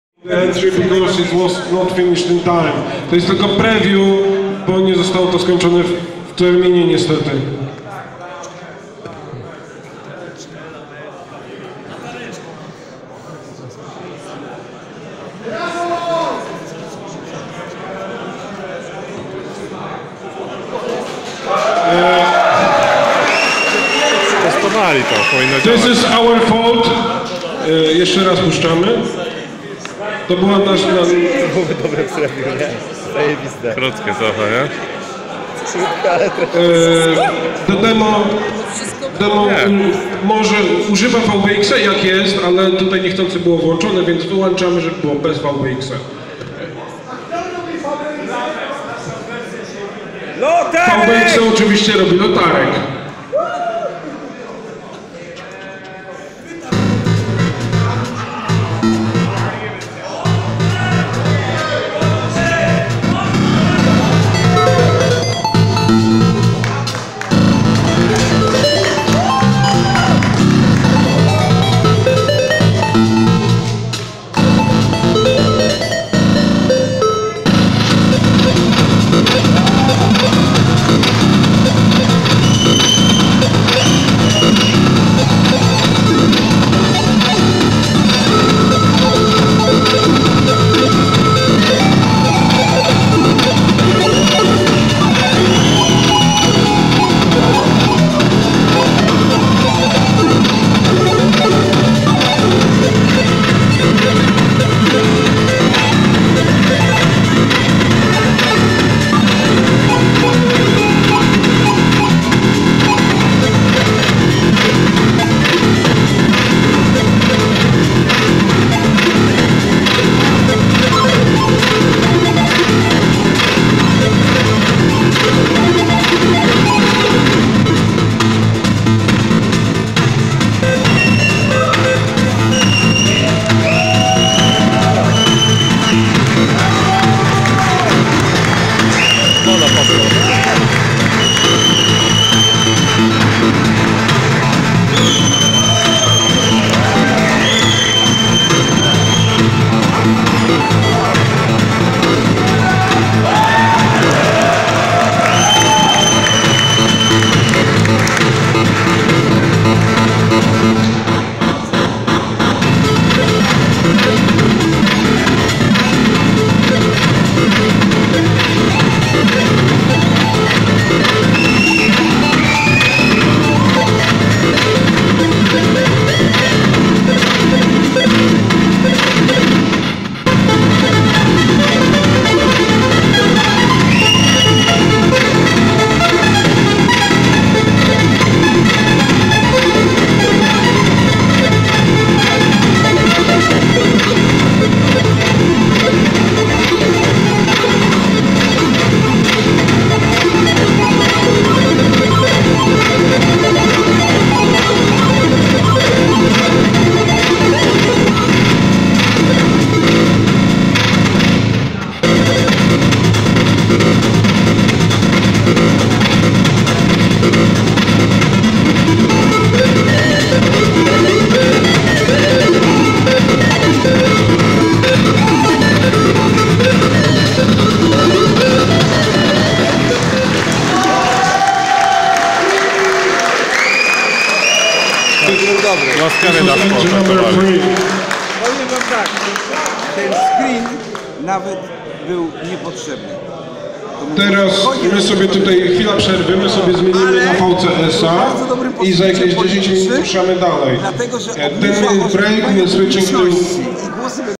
Prozac Dream / Lamers - Atari XE/XL Demo preview - PARTY VERSION
Our demo preview took second place on SillyVenture 2016.
Unfortunately, It wasn't finished on time.